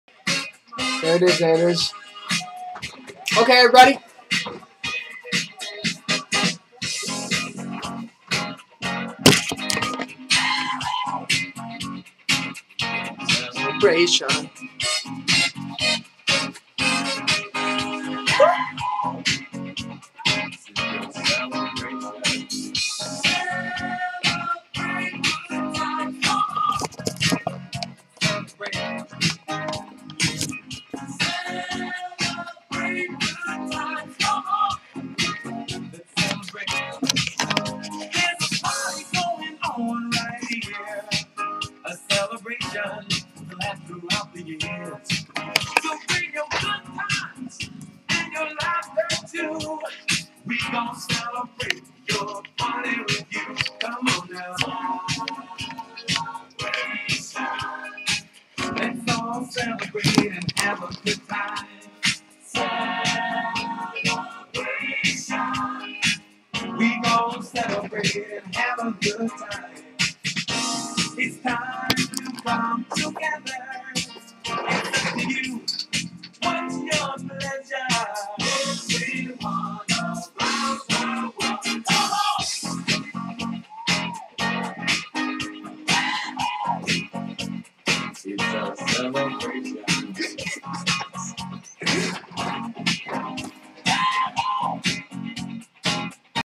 Kickstarter 1000$ Dance Video!
Thank you everyone who donated to the project, I really appreciate all the positive things said about the project, I did not think we would raise 1000$ over the past three days but we took a leap of faith and WE DID! Here is me showing my gratitude cause I am STOKED.